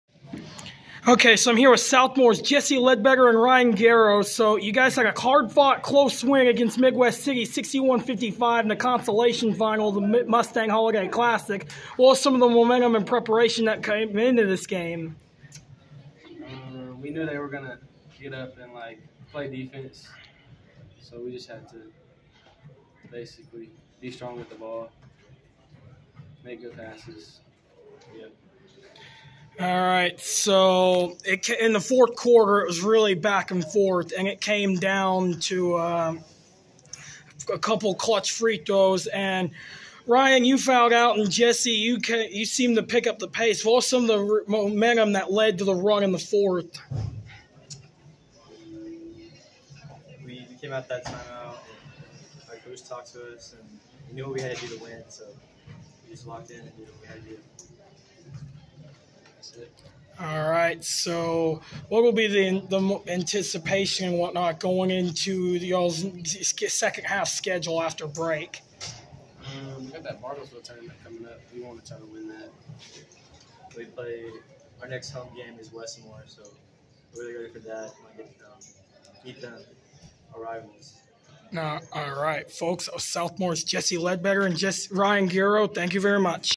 Interview with Jesse Ledbetter and Ryan Guerrero of Southmoore
I interview Jesse Ledbetter and Ryan Guerrero of Southmoore after the team's 61-55 win over Midwest City in the consolation final of the Mustang Holliday Classic